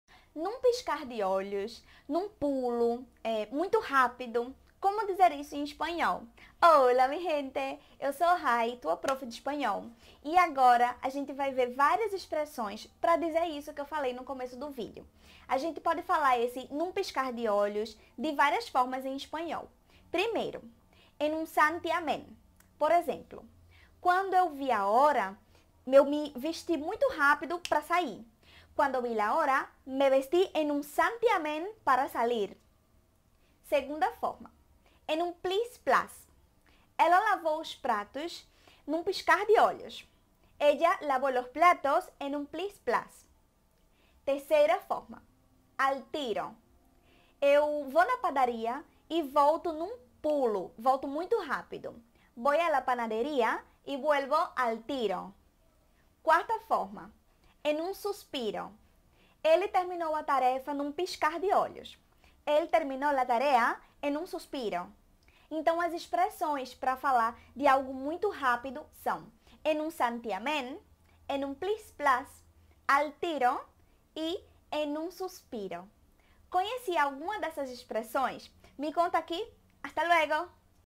4 formas de dizer: Num piscar de olhos
O que você faz num piscar de olhos? de maneira bem rápida?

Se liga nessas 4 maneiras pra aumentar teu vocabulário e escreve uma frase nos comentários para praticar,

¡¡Practica y conéctate!

• Entre para o nosso grupo no Telegram e receba conteúdos exclusivos